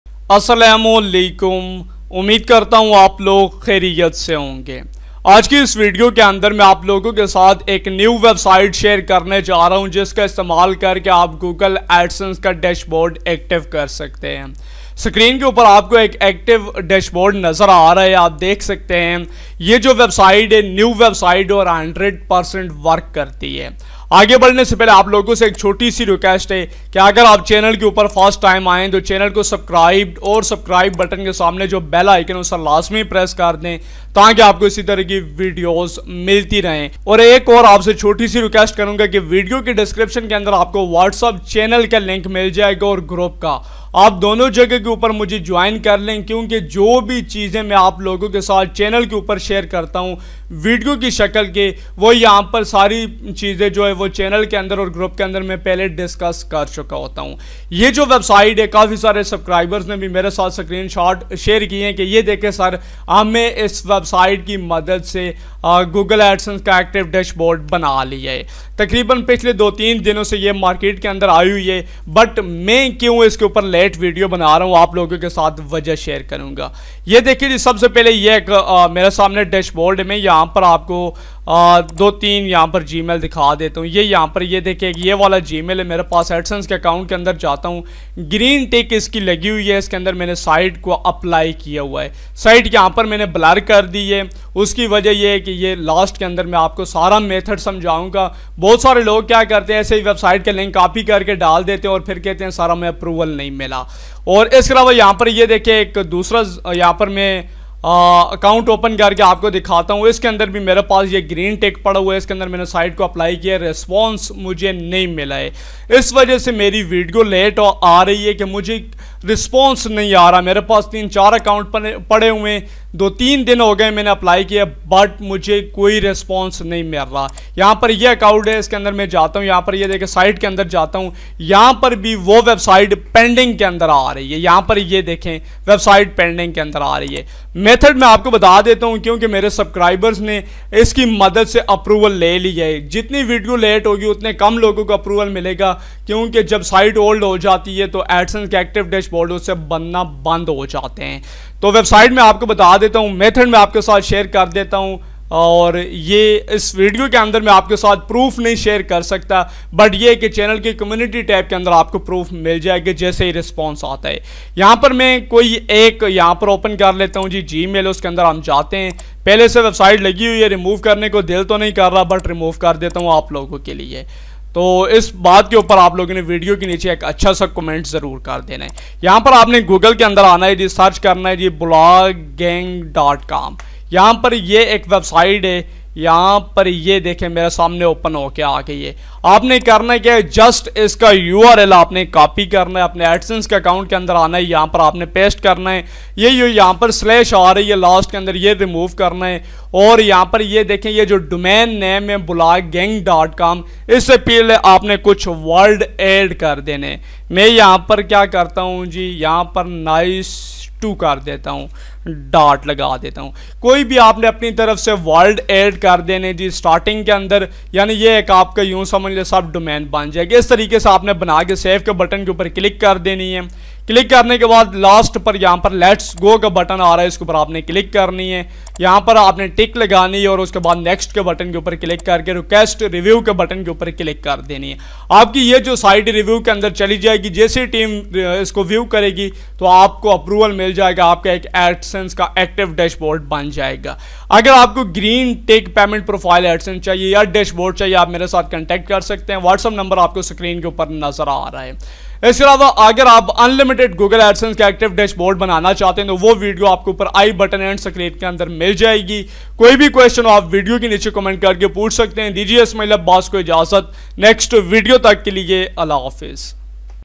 How to Make Google AdSense Active Dashboard 2024 || Google AdSense Active Dashboard Kaise Banaye
Whatsapp Number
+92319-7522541

In this video tutorial you will learn about following topics: -
1. How to Make Google AdSense Active 2024
2.  AdSense Active Dashboard Kaise Banaye
3. Google AdSense Active Dashboard 2024